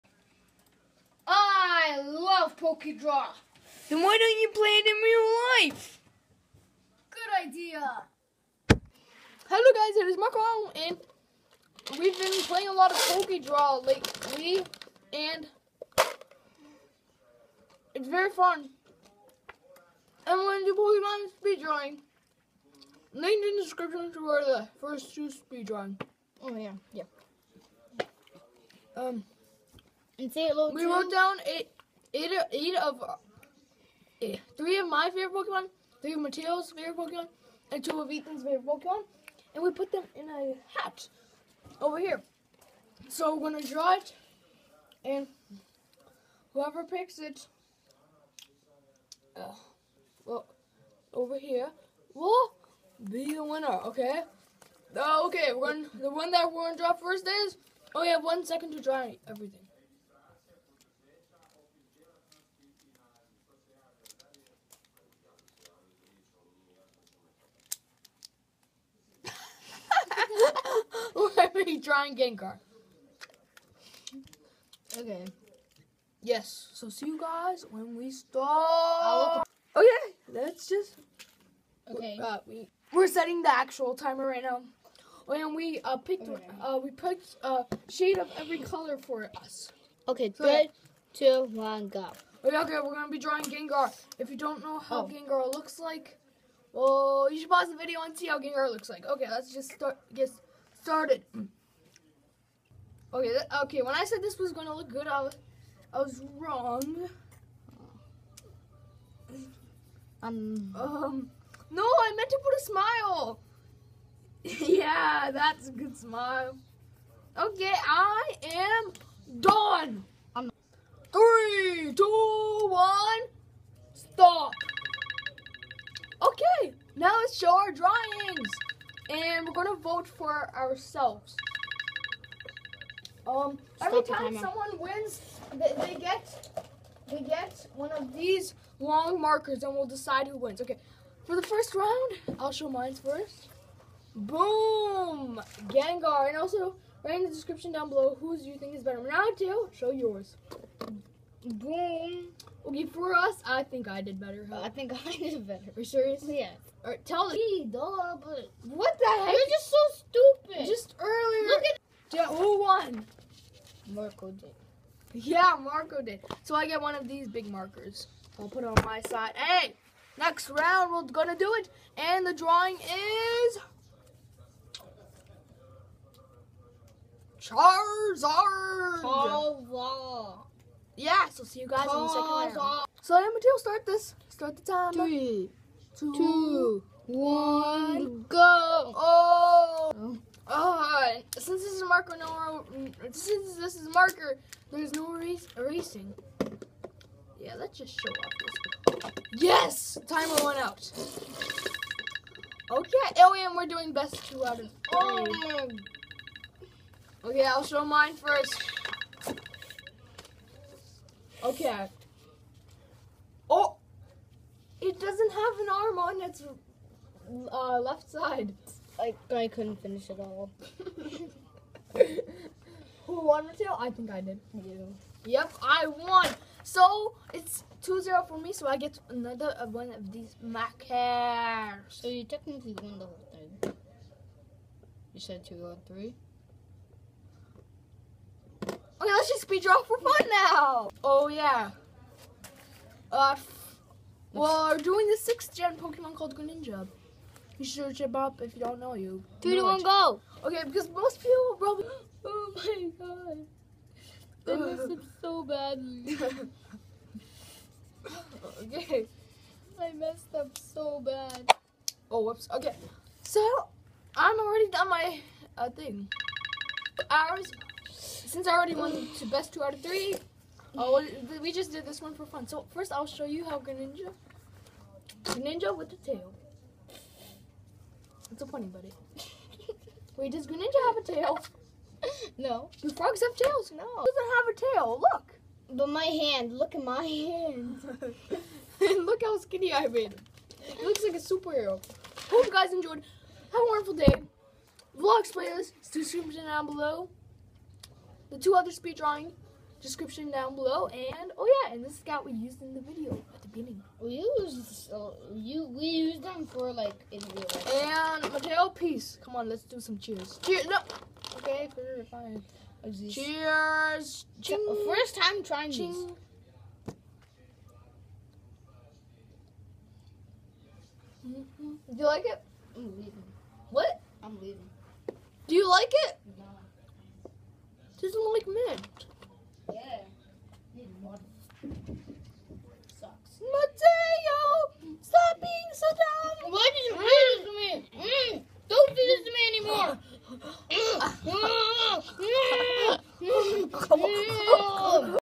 PokeDraw | Living Room Edition
Today we are speed drawing... POKEMON!!!

Hope you enjoy and have a wonderful day :D